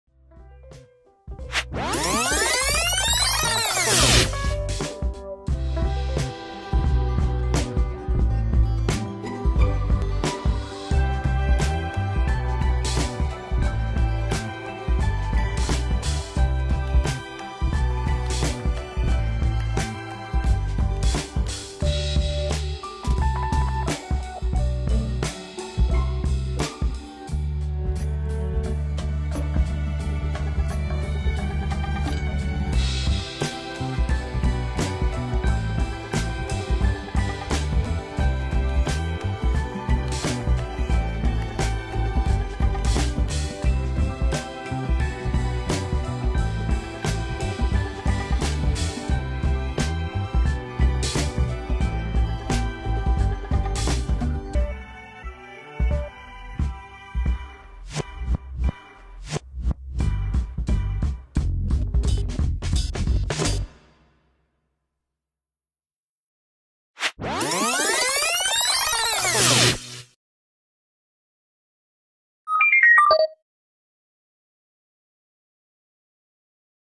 Work in the style of Philippe Baudelocque (pt1)
A quick look at the style of French street artist Philippe Baudelocque, and making patterns